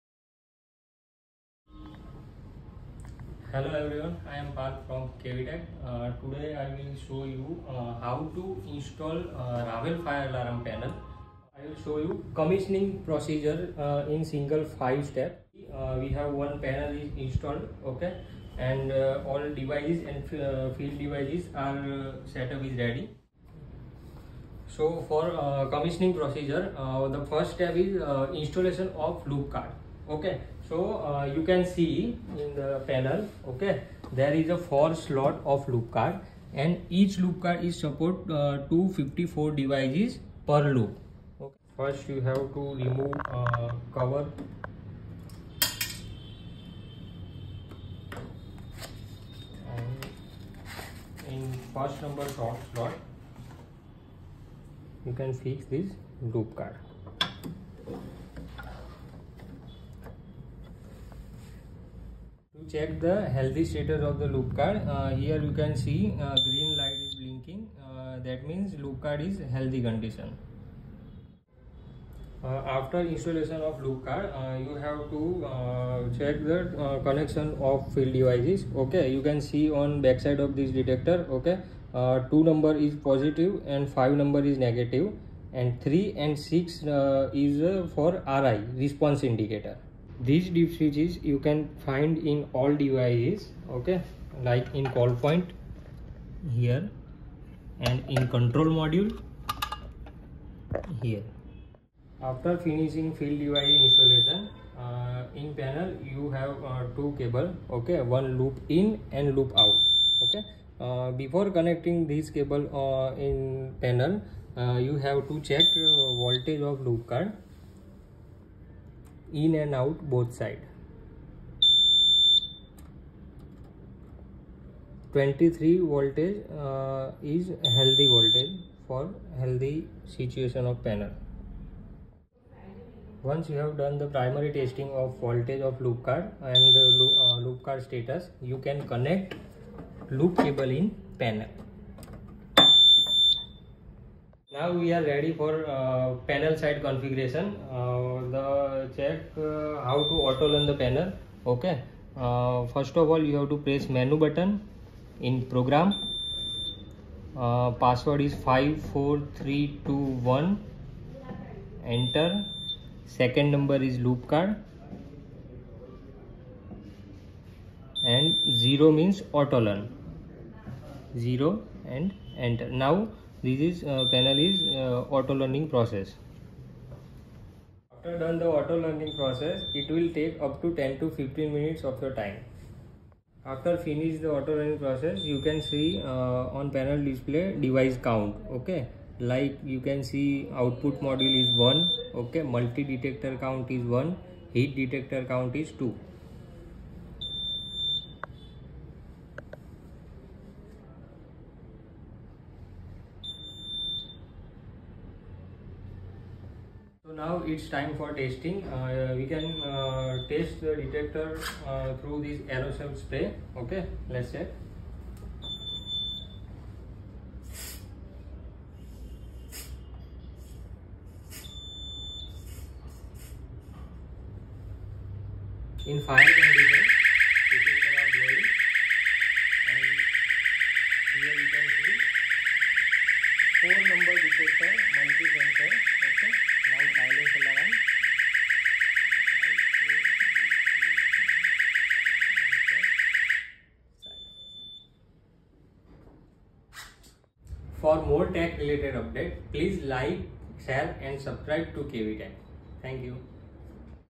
Ravel Avani Fire Alarm Panel- Configuration & Testing- Cavitak
Fire Alarm Panel Configuration & Testing - powered by Cavitak.

UL listed – Single Loop Expandable Up To 4 Loop Analogue addressable fire alarm panel with 254 devices of any combination in a loop,1016 devices of any combination in a panel, Networkable upto 8 panels in a node. Tocuh key panel with USB inbuilt for Laptop connectivity.Front panel programmable with Loop redundancy.